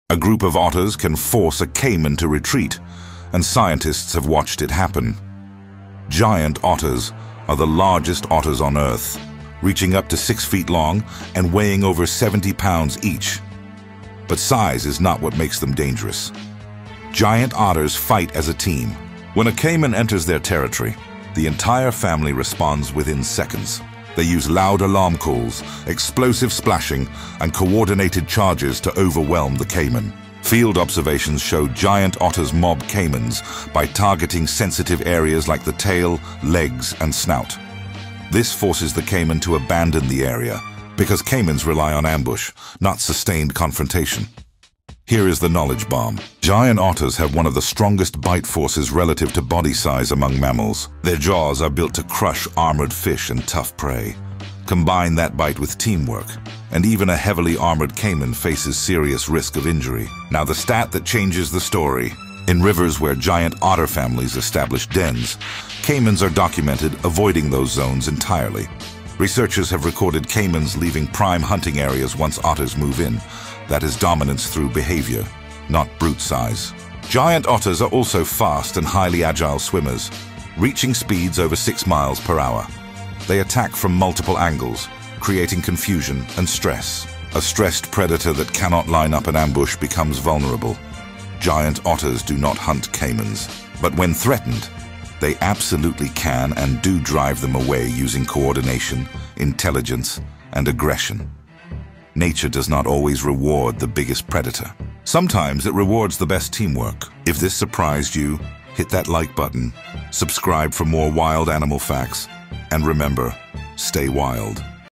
😱Otters Gang Up and Kill a Caiman🦦☠️🐊
Giant otters have been documented attacking caimans as a group, targeting sensitive areas and causing serious injuries. In rare cases, researchers and video evidence show caimans being overwhelmed during territorial defense. This video explains how teamwork, speed, and aggression allow otters to challenge much larger predators.